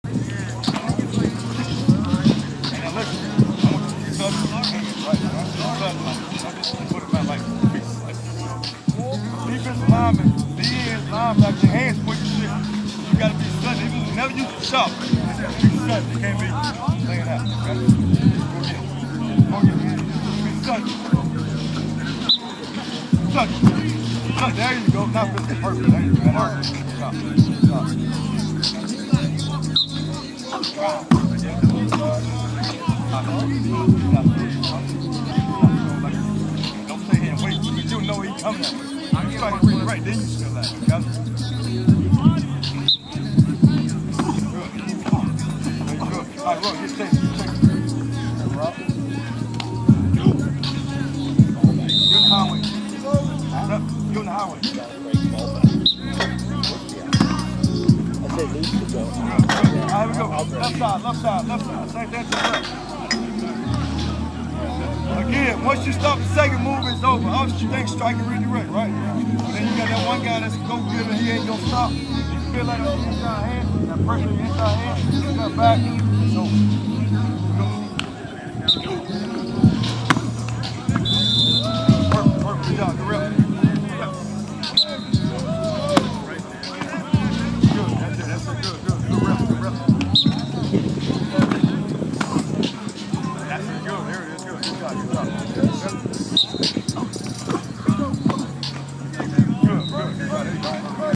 BlueGoldNews.com: WVU Football Running Back Blocking II 8/9/21
West Virginia running backs drill on blocking and rush pickup